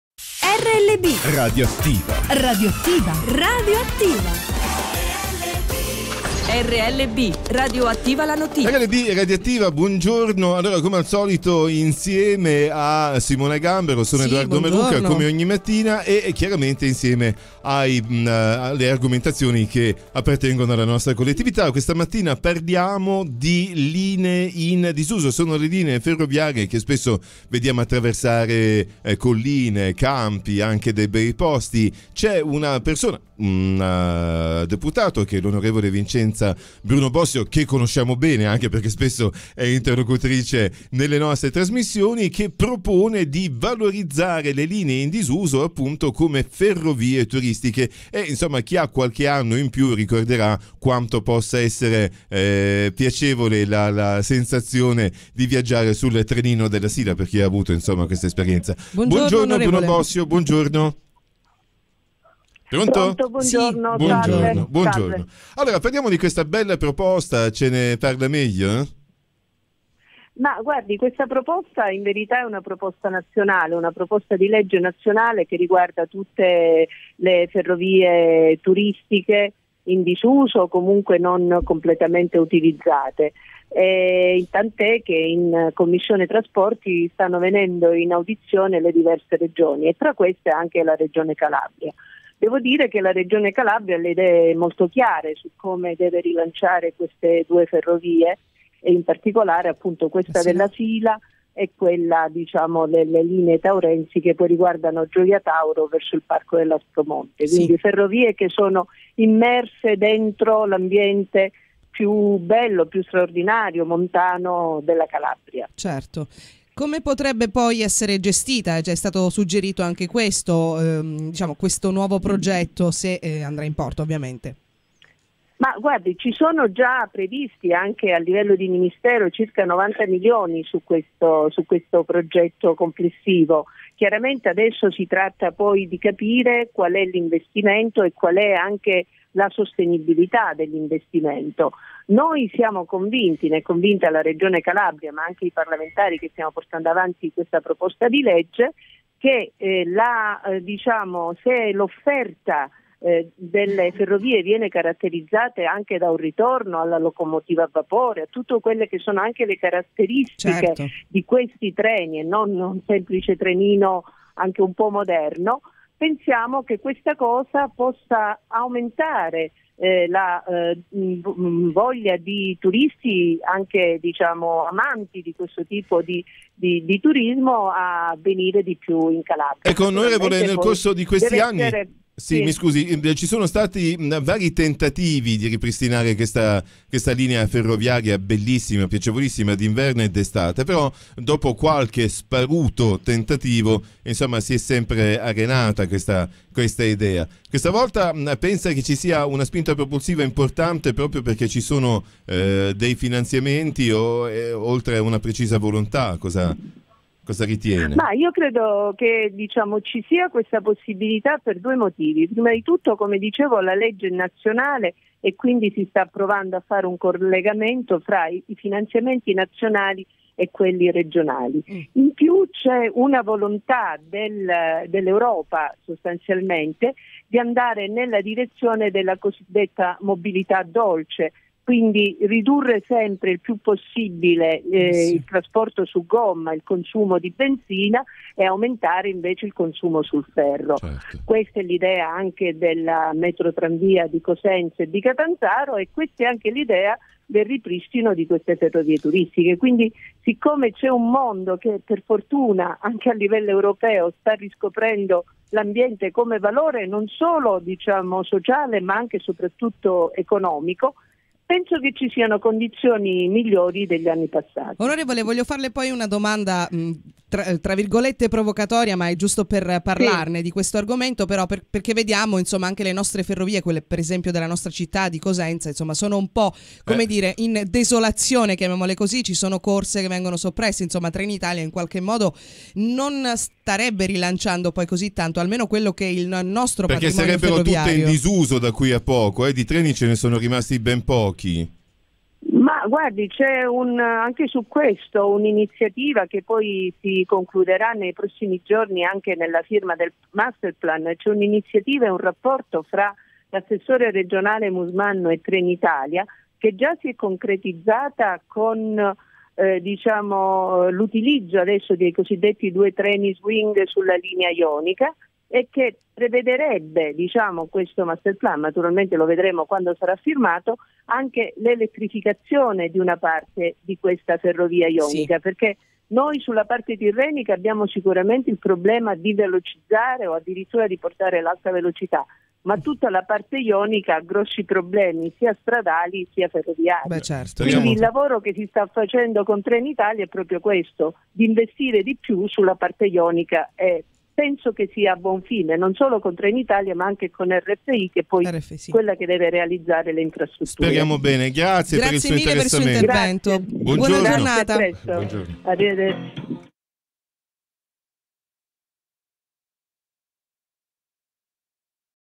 enza bruno bossio
l'On Enza Bruno Bossio parla della proposta di legge per la valorizzazione delle linee ferroviarie in disuso e della riconversione ad uso turistico_ascolta_